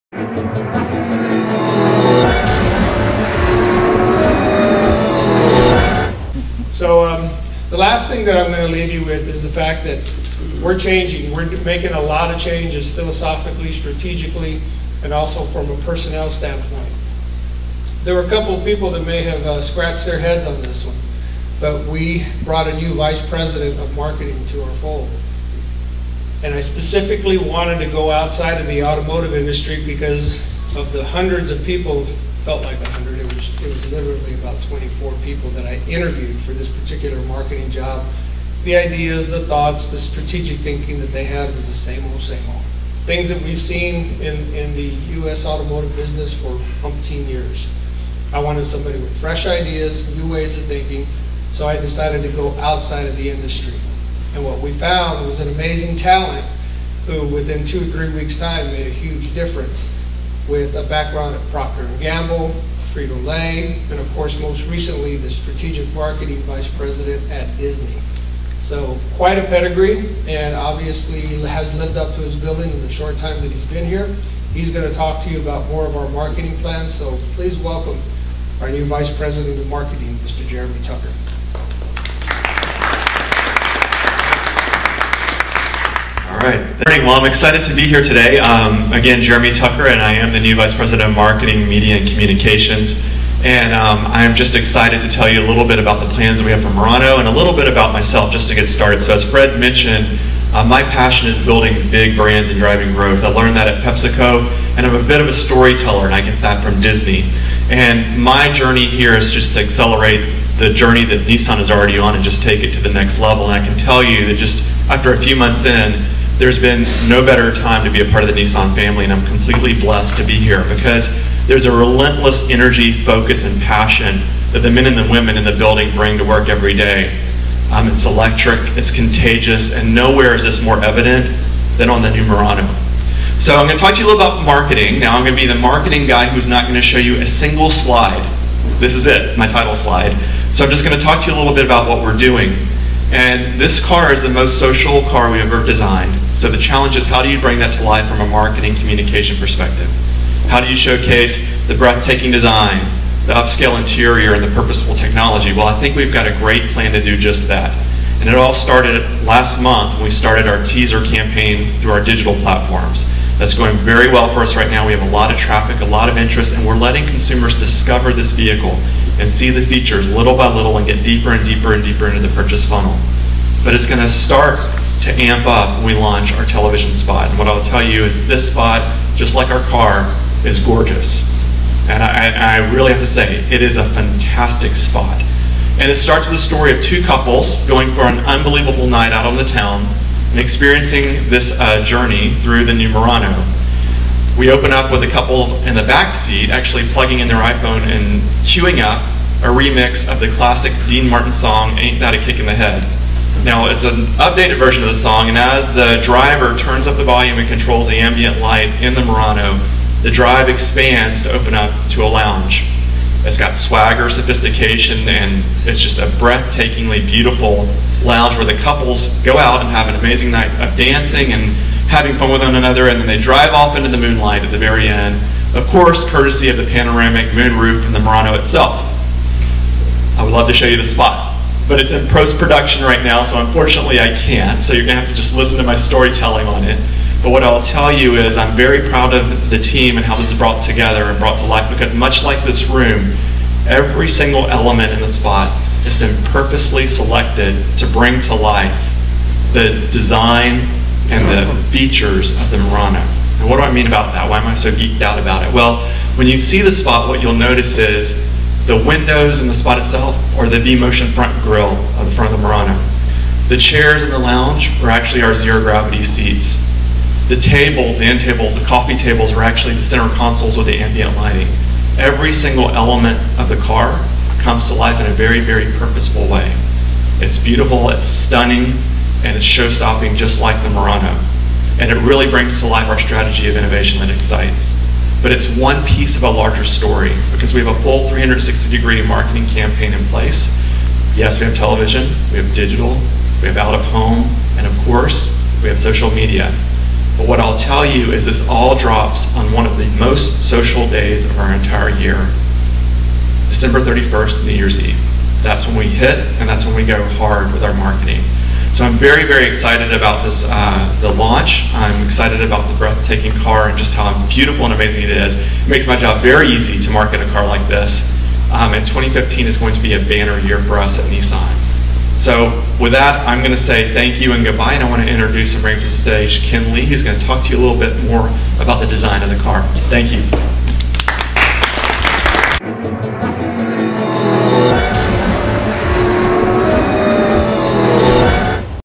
Jeremy Tucker, new VP of Marketing at Nissan USA at the 2015 Nissan Murano Press Event
Jeremy Tucker, new Vice President of Marketing Communications and Media at Nissan USA at the 2015 Nissan Murano Press Event.